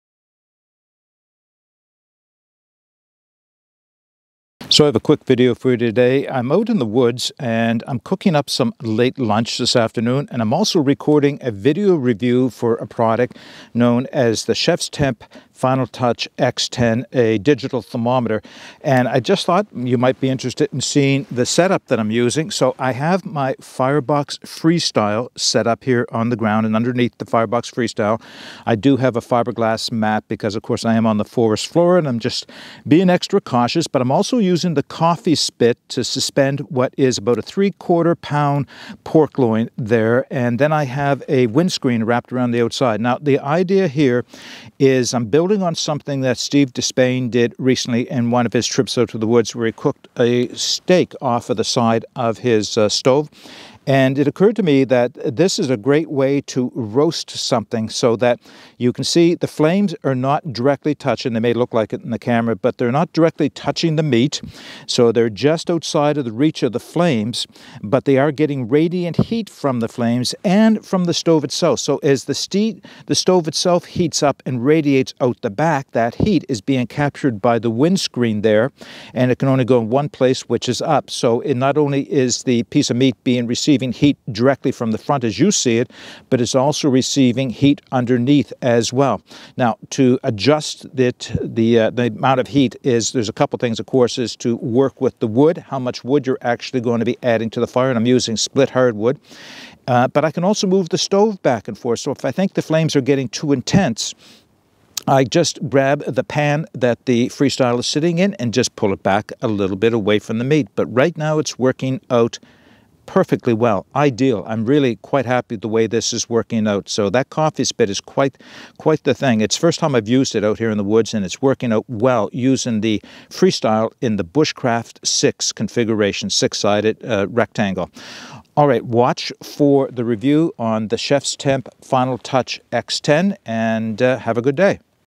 Roasting Pork with the Firebox Freestyle and Coffey Spit
Quick video showing my setup for roasting a pork loin using the Firebox Freestyle and Coffey Spit